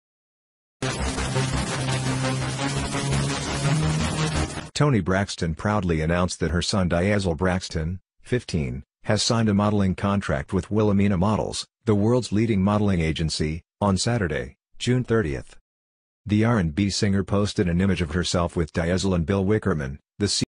TONI BRAXTON'S 15-YEAR-OLD SON SIGNS WITH WILHELMINA MODELS (PHOTOS)
Toni Braxton's 15-Year-Old Son Signs With Wilhelmina Models (PHOTOS) Toni Braxton proudly announced that her 15-year-old son Diezel Braxton signed a modeling contract with Wilhelmina Models, the world's leading model agency, on Saturday (June 30).

The R & B singer posted an image of her with Diezel and Bill Wickerman, the CEO of Wilhelmina, to share the exciting news. She subtitled the photo, "Congratulations Diezy @ tenel.braxton for signing with the Wilhelmina Modeling Agency!" With a dazzling emoji and a black heart.

Diezel is Braxton's second son with her ex-husband, Keri Lewis. Diezel was diagnosed with autism from an early age, which led his mother to actively participate in organizations such as Autism Speaks. She was named global ambassador of the defense group in 2012. Two years ago, during an interview, the singer revealed that her son, who was then 13 years old, no longer exhibited symptoms of the autism spectrum.

Diezel shared the news on her own Instagram, subtitling the image, "It's time to get to work. You're ready @wilhelminamodels."